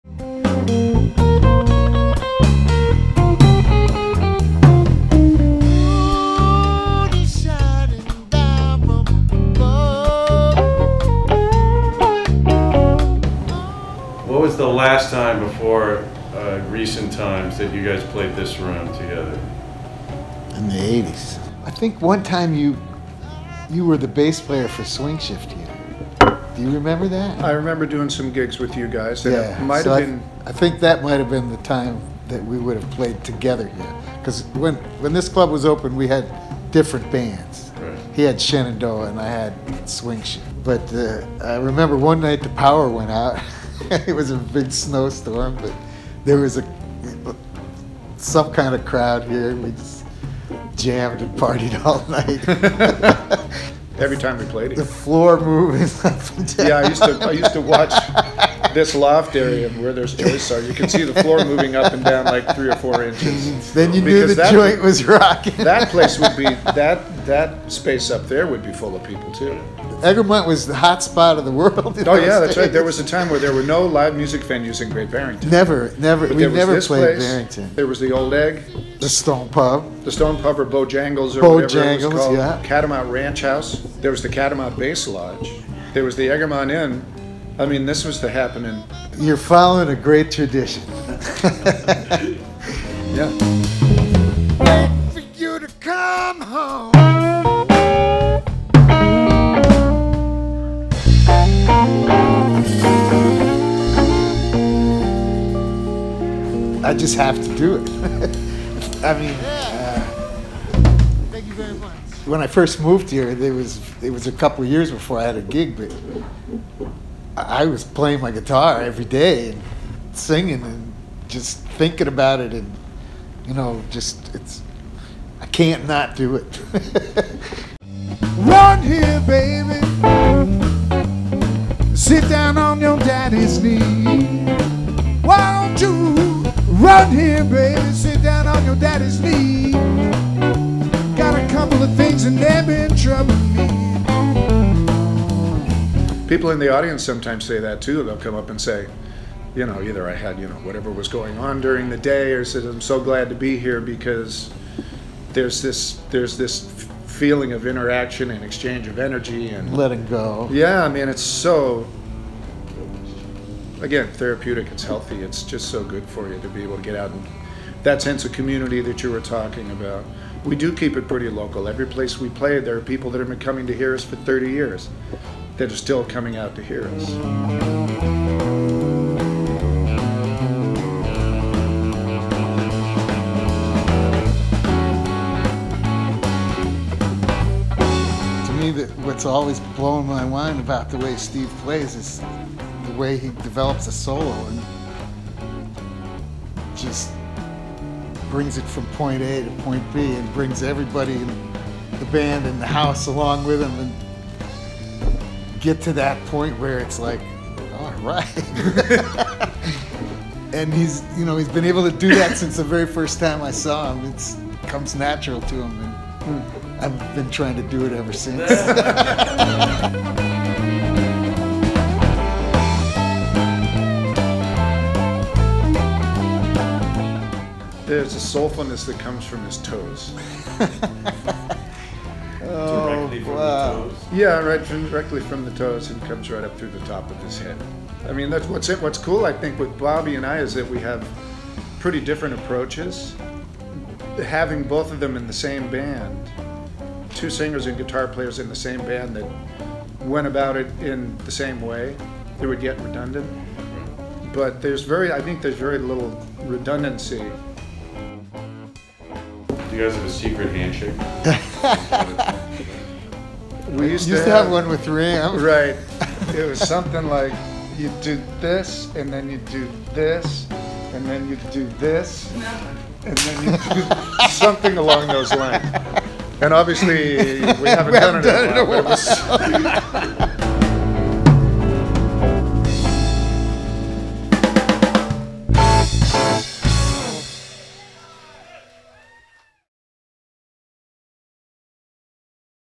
Barn Mini-Docs: Bobby and Steve
Check out this clip from our ongoing mini-doc series. Talking with Steve Ide and Bob McVeety of the BTUs about music, life and secret handshakes through their almost 40 years of collaboration in the Berkshires and beyond.